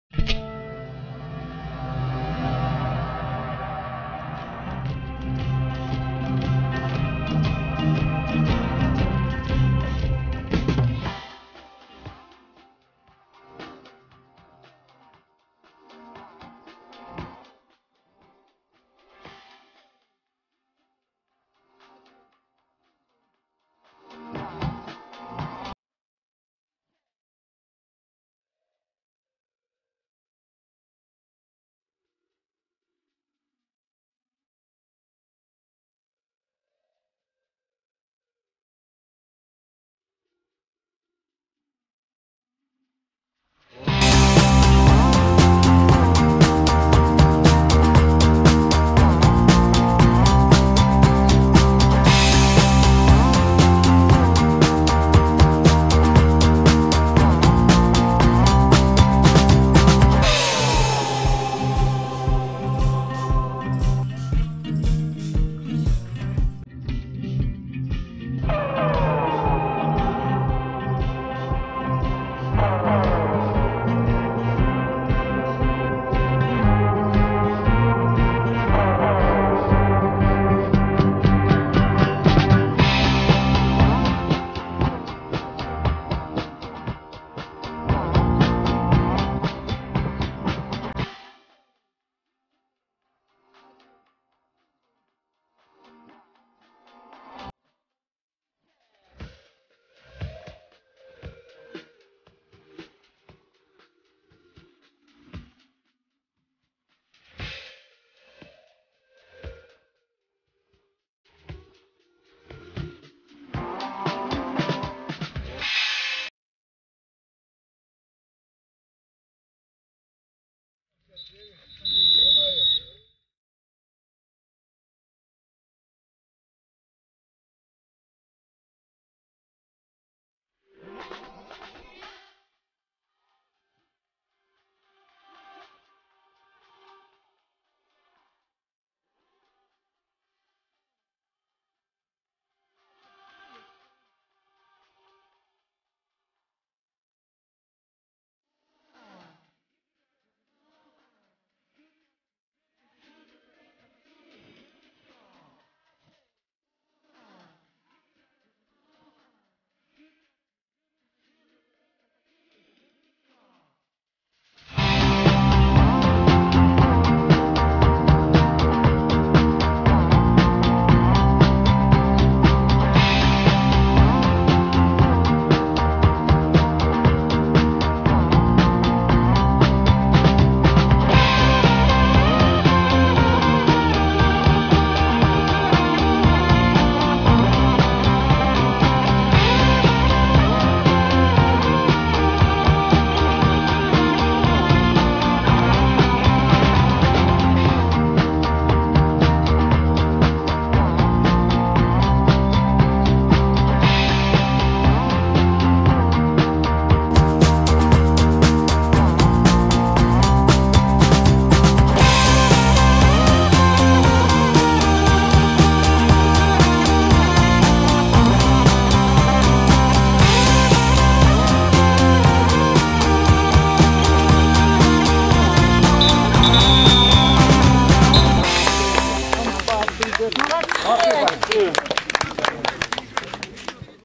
Вторая лига. Зона Юг. "Темп" - "Энергия" (Обзор матча)
Вторая лига нижегородской области по футболу.
Сезон-2015. Зона "Юг". 6-й тур.

"Темп" (Первомайск) - "Энергия" (Дальнее Констатиново) - 4:1